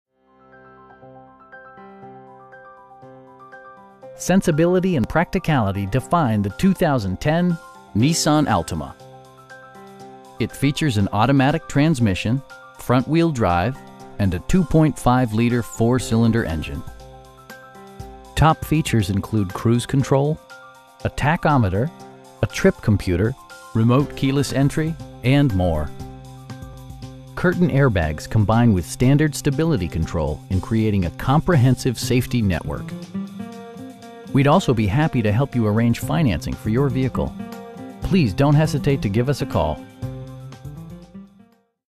2010 Nissan Altima 2.5 S in Dedham, MA 02026
Sensibility and practicality define the 2010 Nissan Altima. It features an automatic transmission, front-wheel drive, and a 2.5 liter 4 cylinder engine. Top features include cruise control, a tachometer, a trip computer, air conditioning, power door mirrors, remote keyless entry, an overhead console, and more. Curtain airbags combine with standard stability control in creating a comprehensive safety network. We'd also be happy to help you arrange financing for your vehicle. Please don't hesitate to give us a call.